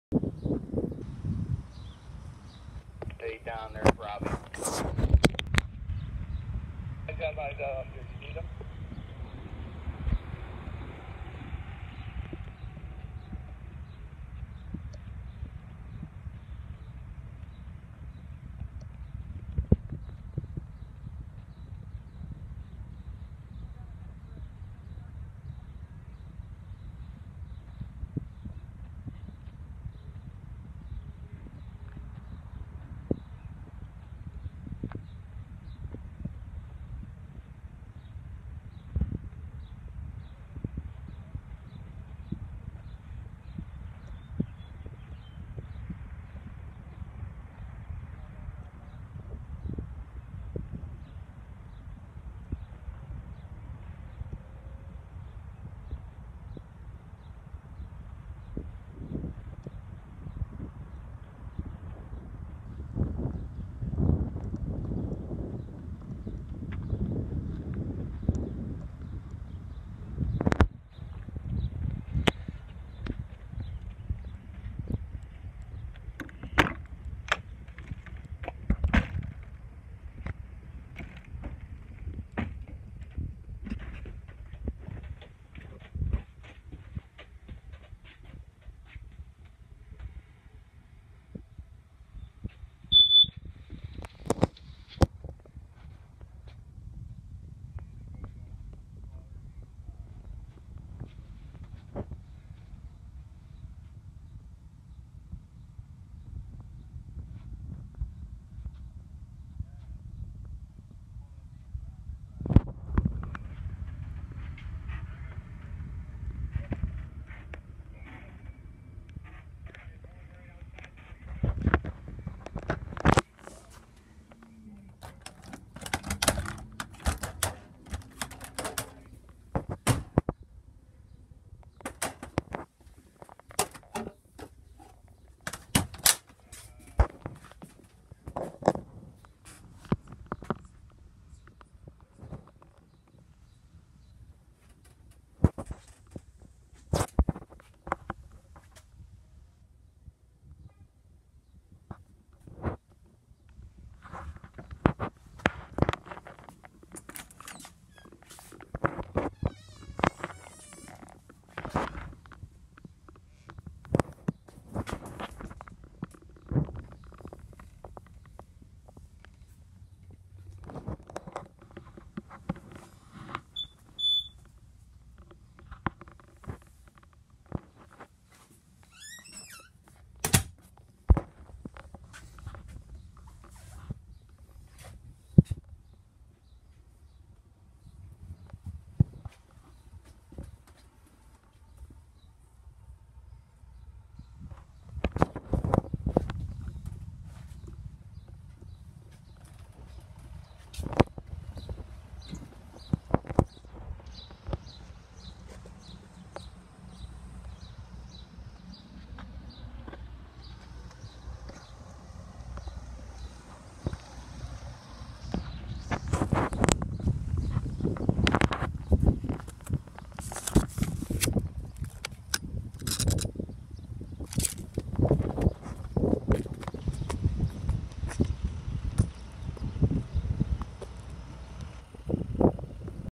2013 KZ Sportsman 272 Trailer | For Sale | Online Auction at Repocast.com
2013 KZ Sportsman 272 Trailer | For Sale | Online Auction at Repocast.com

Scattered small dents with some minor scratches.No evidence that unit was winterid. No battery.